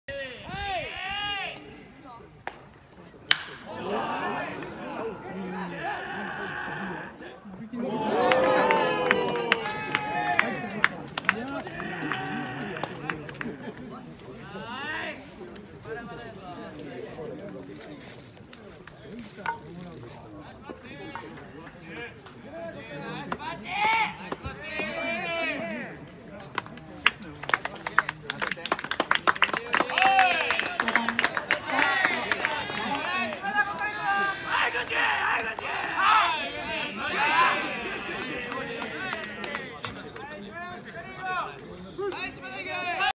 慶應大 郡司裕也（3年 仙台育英）バックスクリーンへのホームラン
2018年2月24日 横浜商科大戦に３番キャッチャーで出場。
次の打席でもレフトへ２打席連続のホームラン。